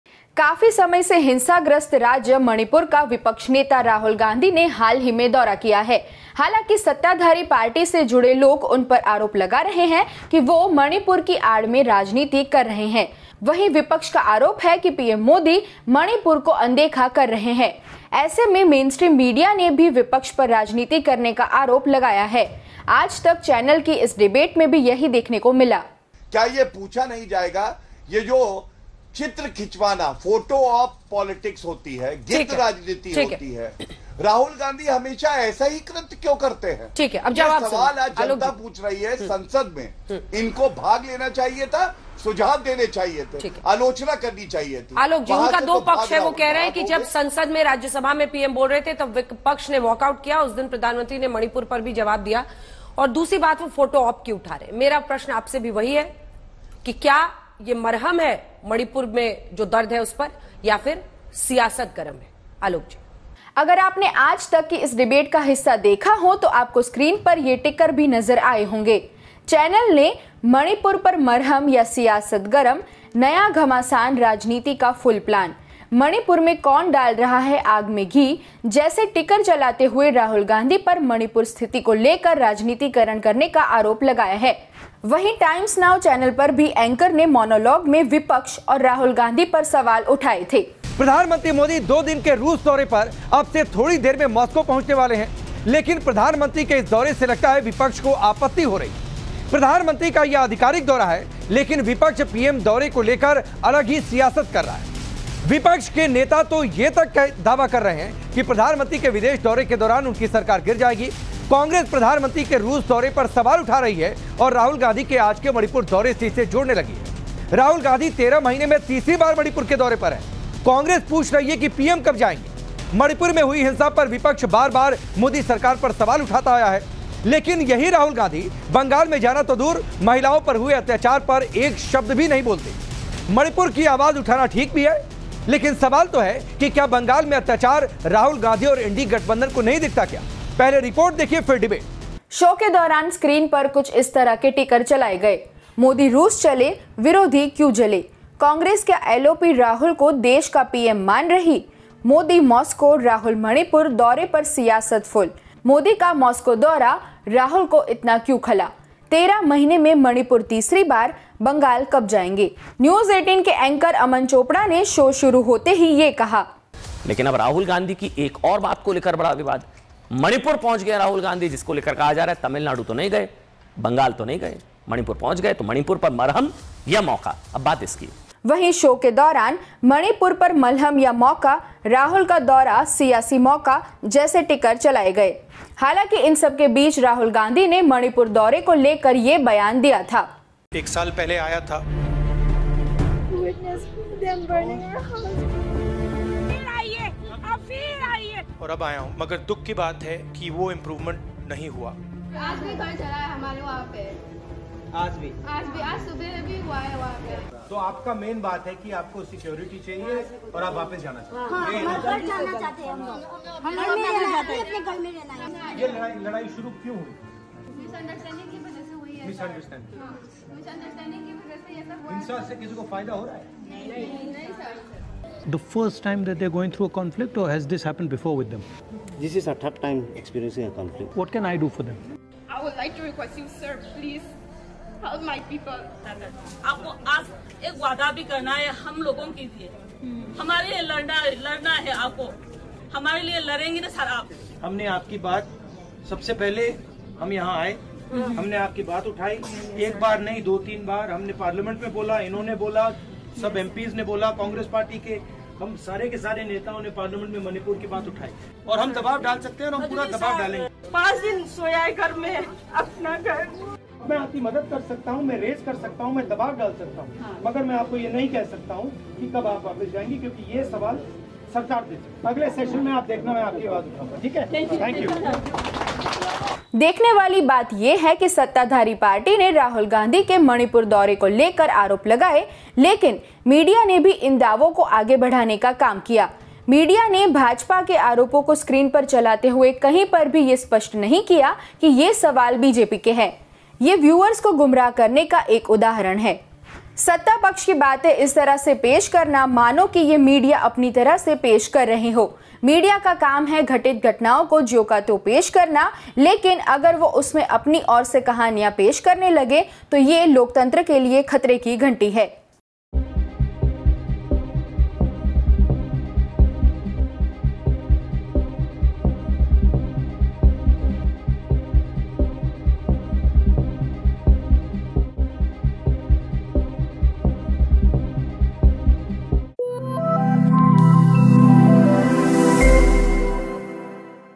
Rahul Gandhi के मणिपुर दौरे को सियासी खेल बतानेवाले मीडिया की कथित रिपोर्टिंग का Decode
Several Hindi news channels echoed the BJP's criticism of Rahul Gandhi's Manipur visit and ran tickers like 'Who's adding fuel to the fire in Manipur' without attribution, presenting them as their own editorial view.